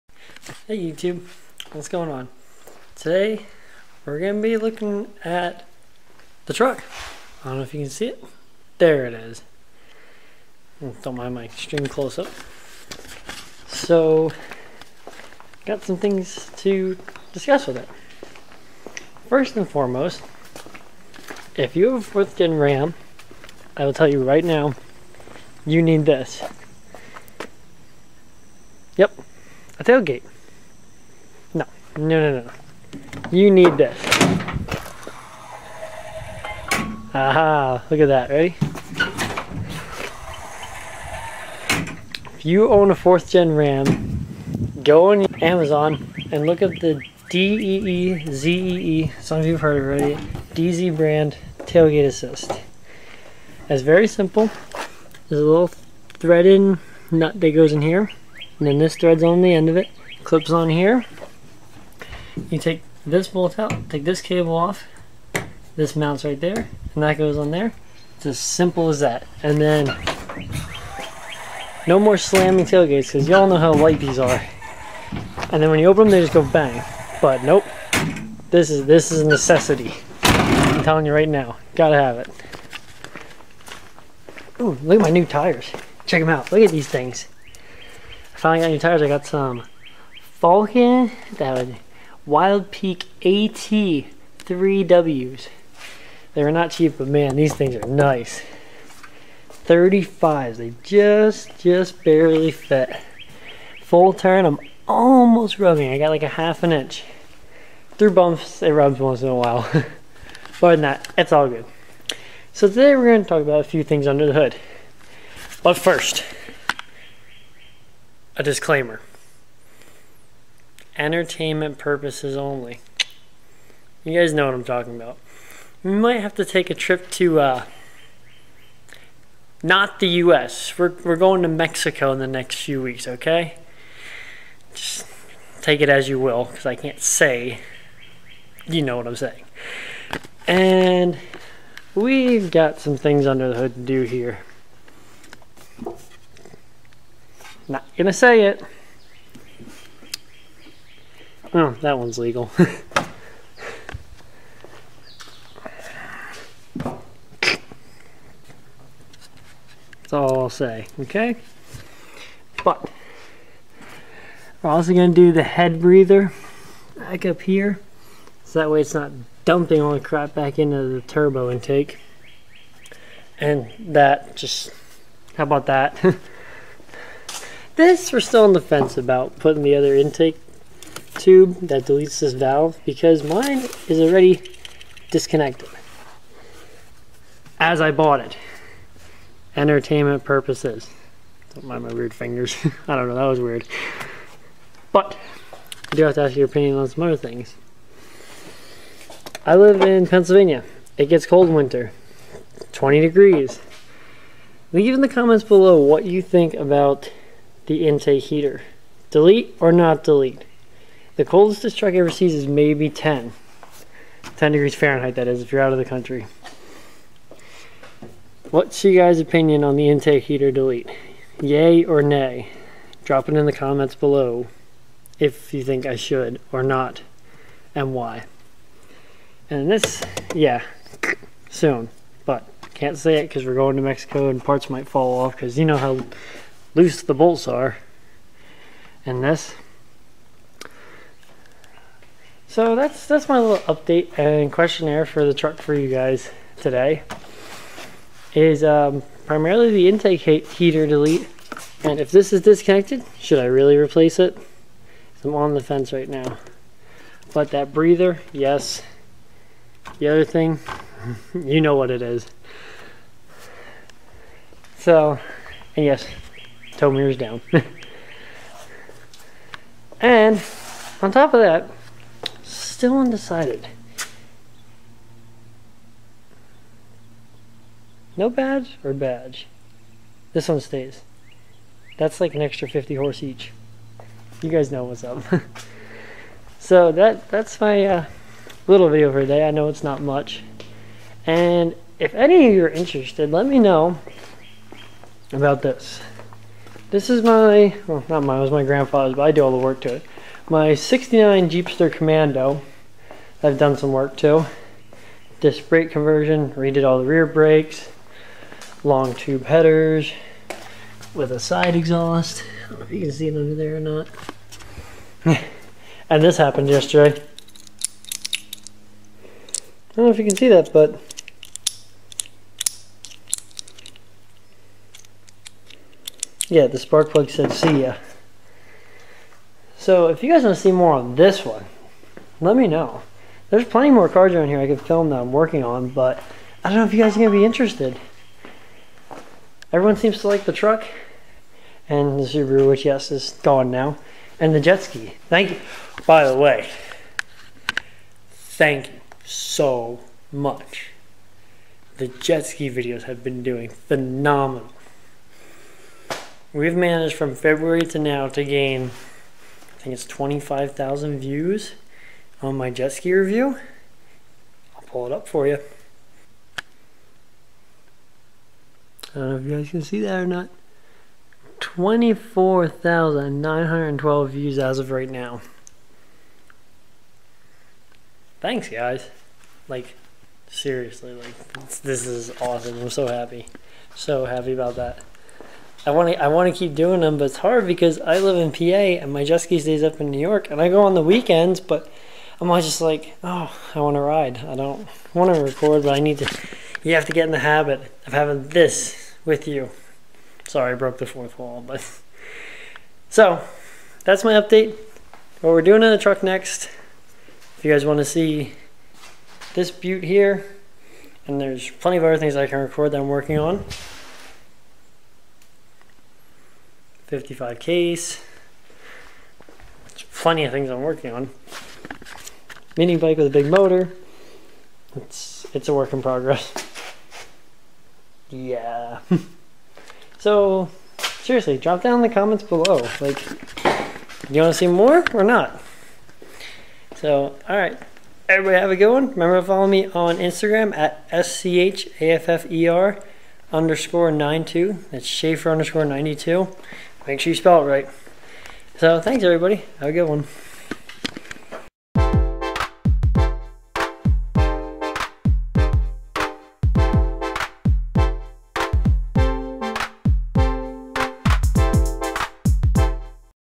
You NEED this if you have a 4th gen Dodge Ram & Cummins g56 updates
Showing you one thing you absolutely need for a 4th gen ram and some things to come for the Cummins g56!